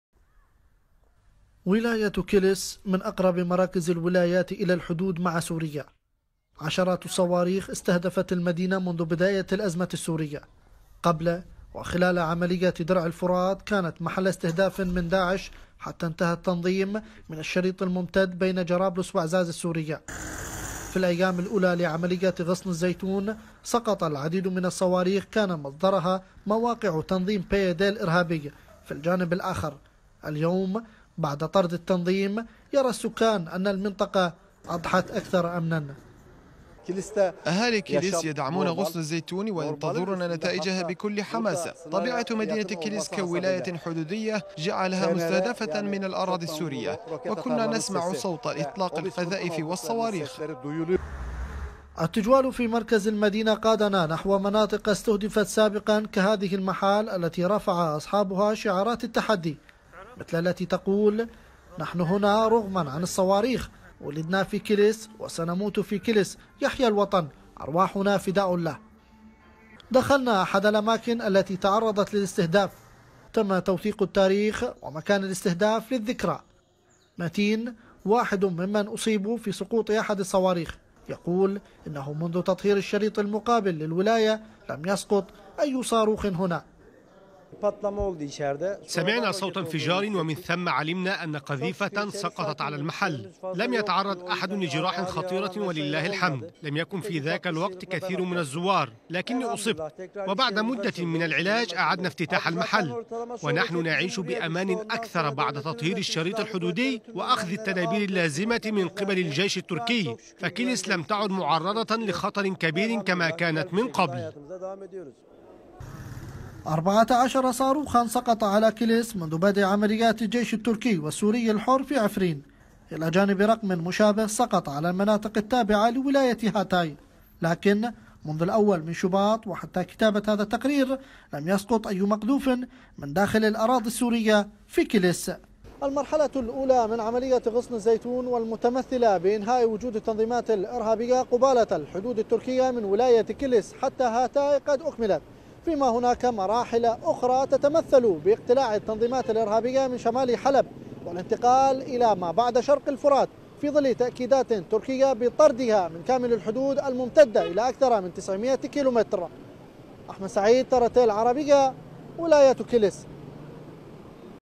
شعور بالارتياح في ولايتي كليس وهطاي بعد تطهير المناطق المحاذية لها شمال سوريا ضمن عملية غصن الزيتون
قناة TRT العربية